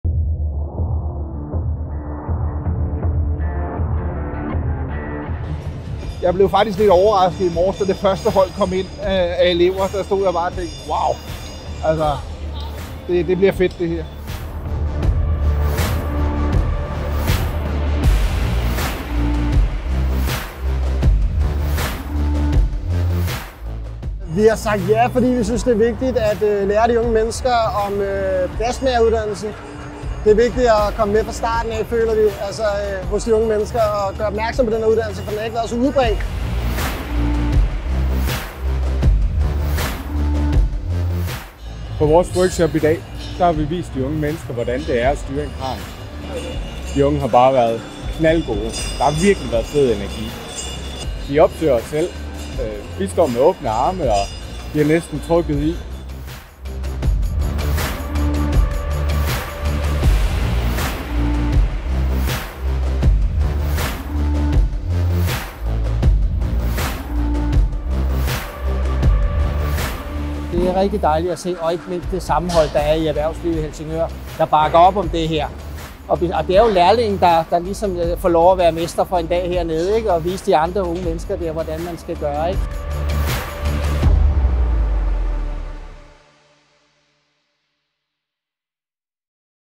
Mini Skills Helsingør 2023 - det siger virksomhederne
Godt 100 mestre, lærlinge og medarbejdere fra mere end 20 lokale virksomheder deltog i Mini Skills Helsingør. Her fortæller nogle af dem om samarbejdet.